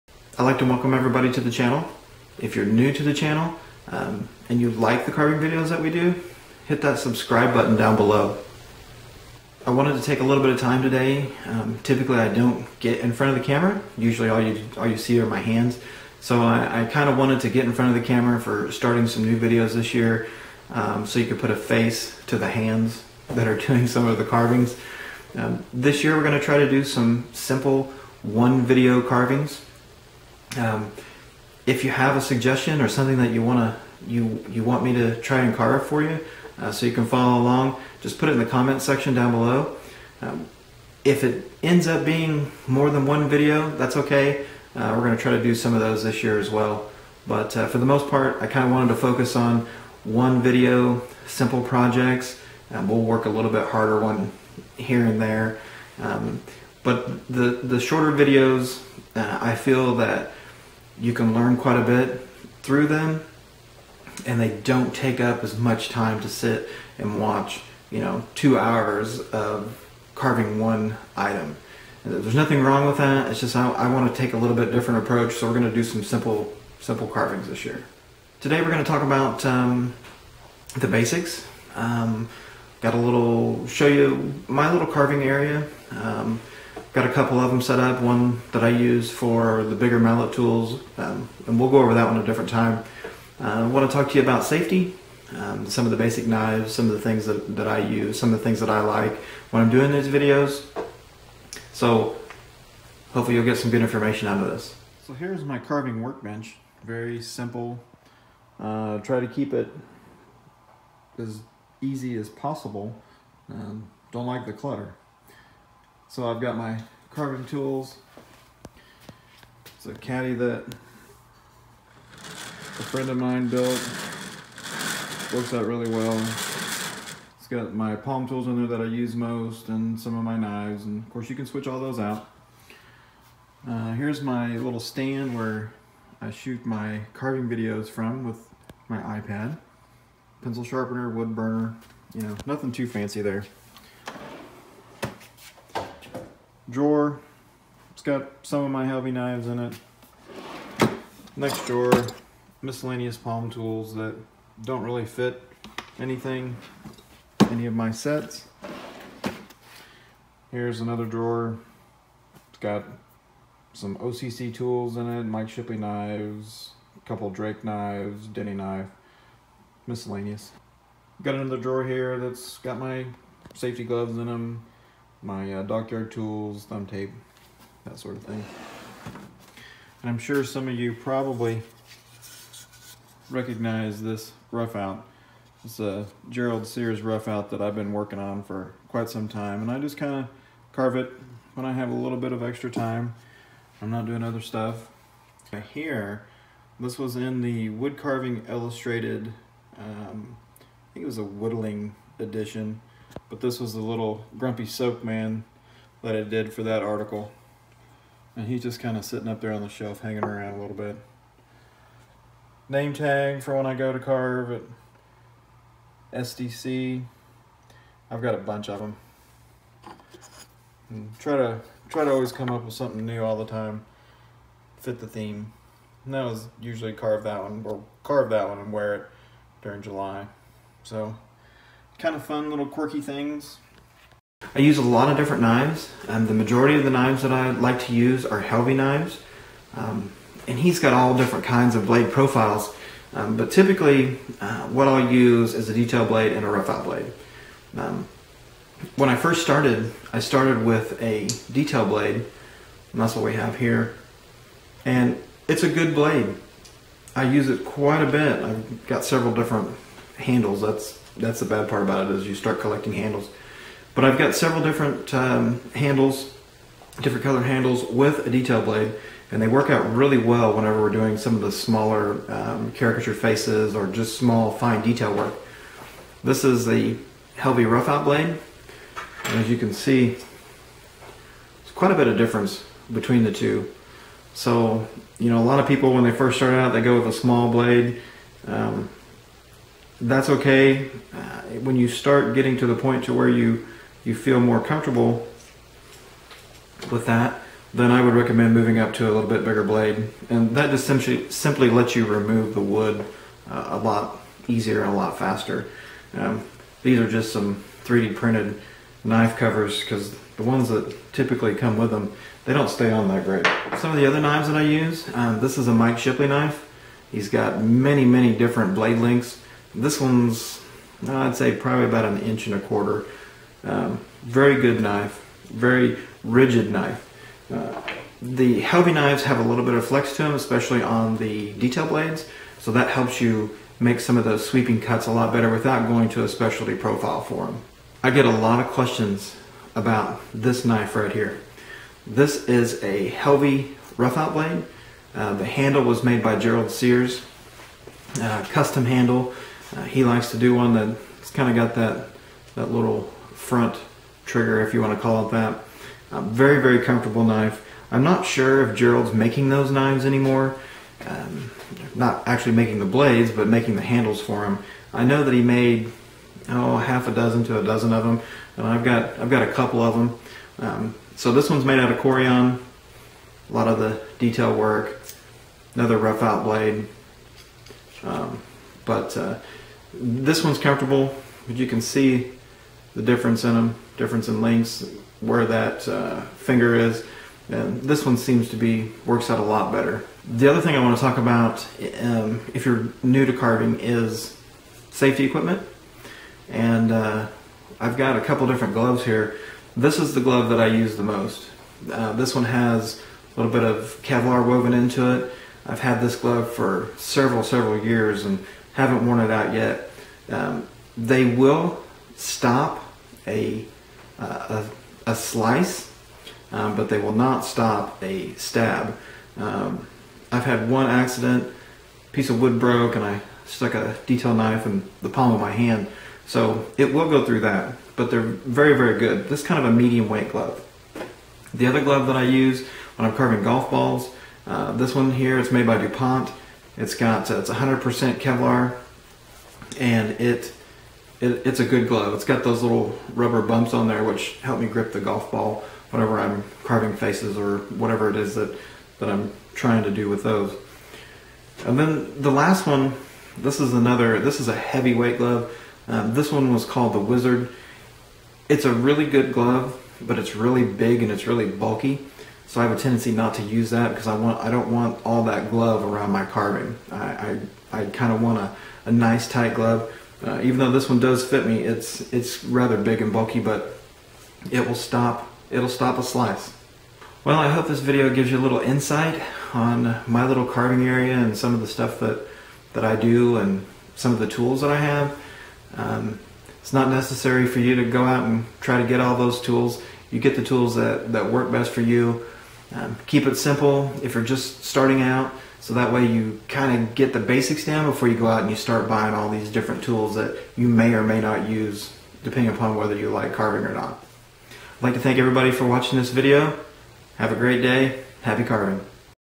My Carving Space and The Basics
Introduction video to carving tools and workspace I use